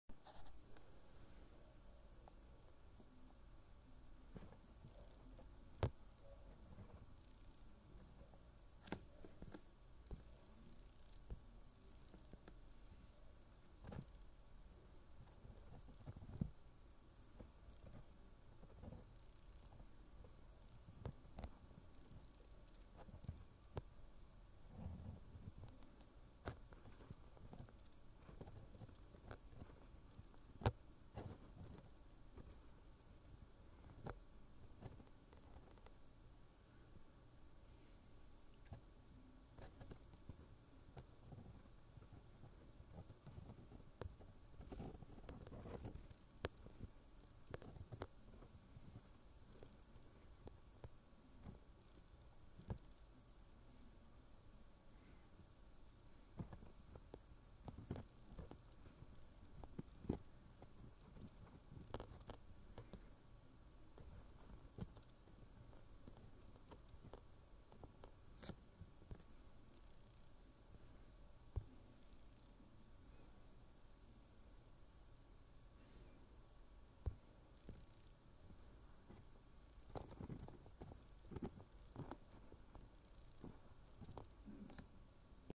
20201226 手機顯微鏡觀察紫錦草表皮細胞
我一開始覺得跟吊竹蘭和蚌蘭差不多
一觀察下果然沒差多少